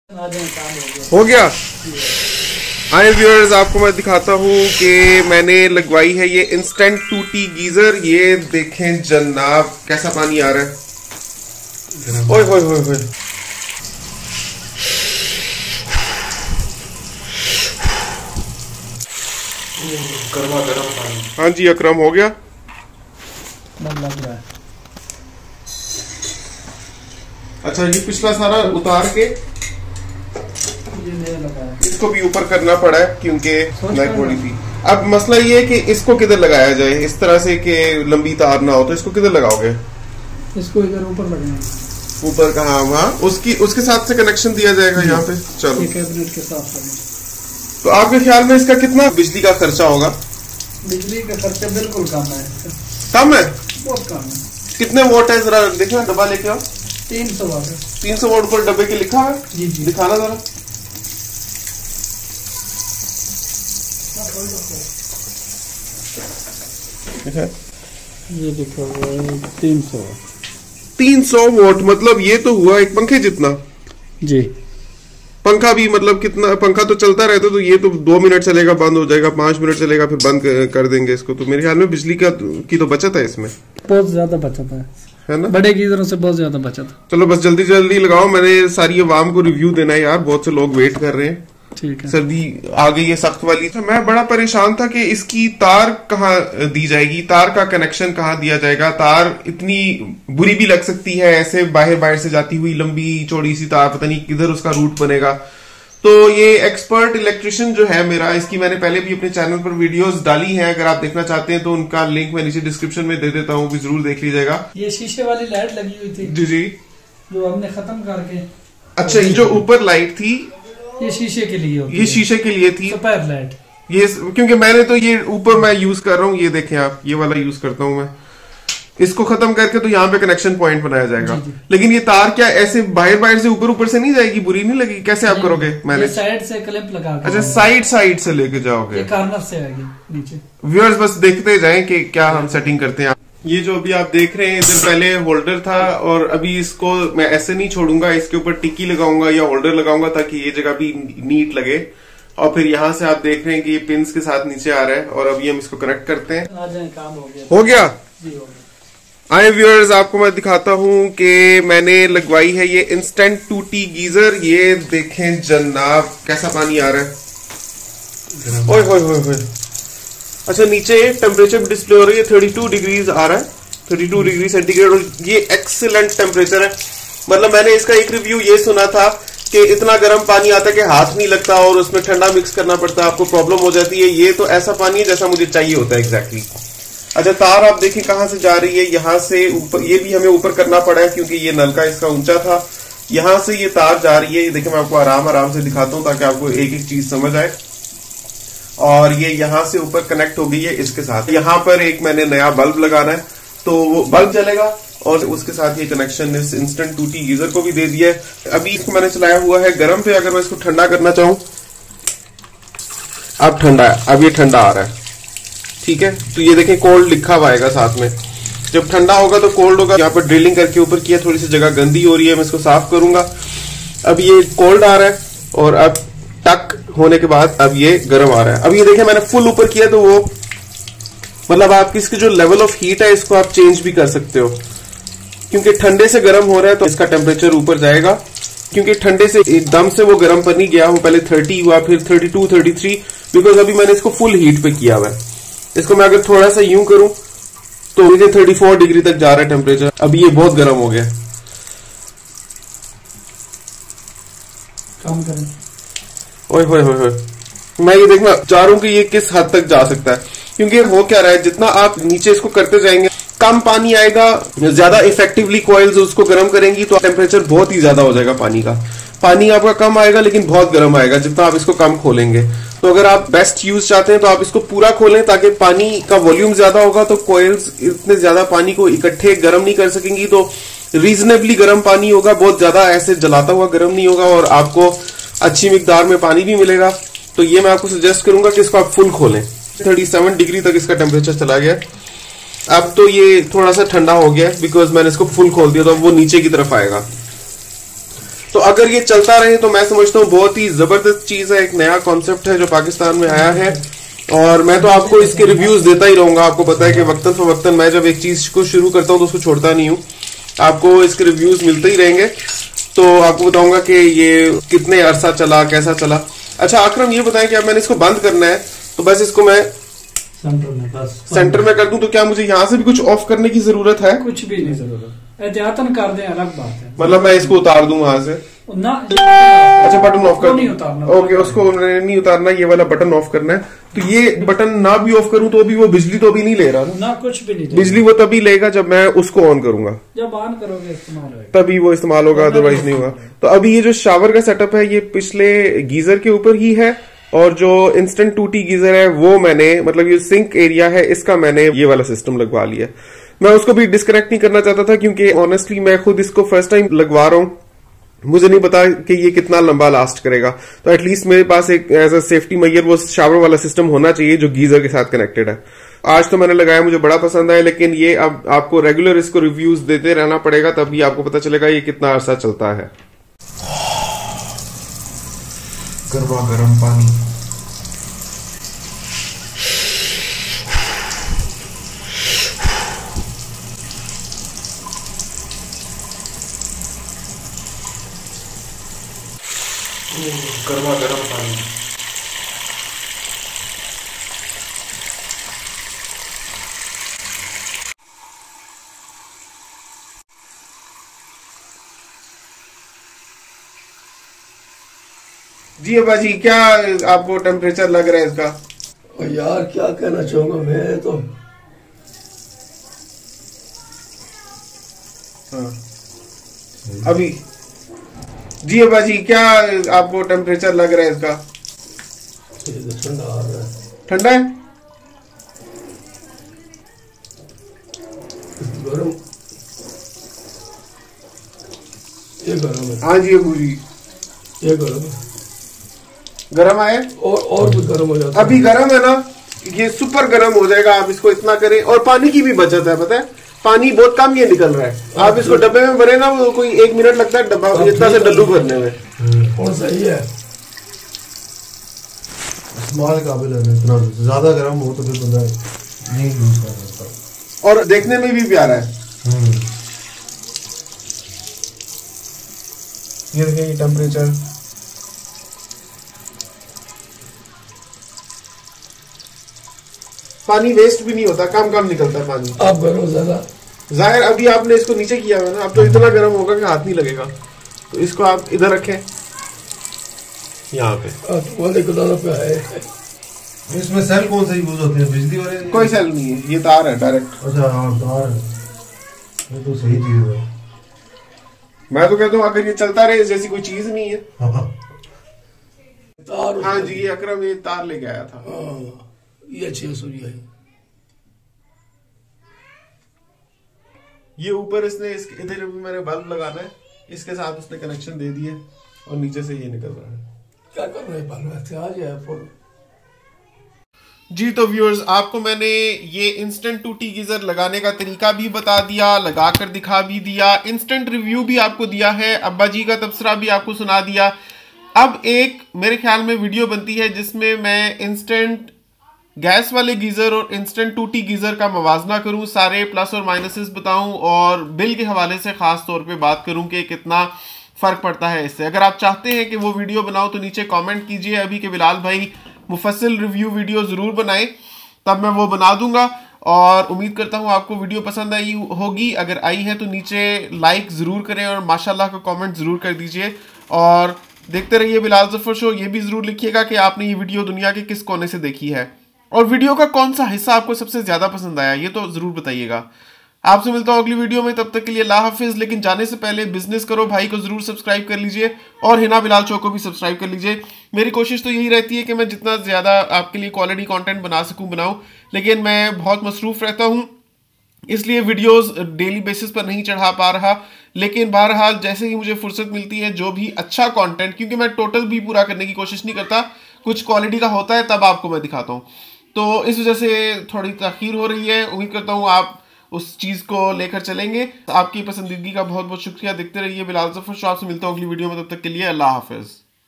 KYA INSTANT TOTI GEYSER LAGWA K GAS AUR BIJLI KI TENSION MUK JAYE GI? انسٹینٹ ٹوٹی گیزر
AAJ MAINE APNE BATHROOM ME INSTANT TOTI GEYSER LAGWA LIYA HAI. DEKHEN KE KAISE INSTANT WATER GEYSER FIT KARTE HAIN. HOT WATER TAP LAGWA KE MAIN PANI KO CHECK KAR RAHA HOON. انسٹینٹ ٹوٹی گیزر
Contact Civil Engineer Bilal Zafar to buy house design or waterproofing chemical at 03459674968.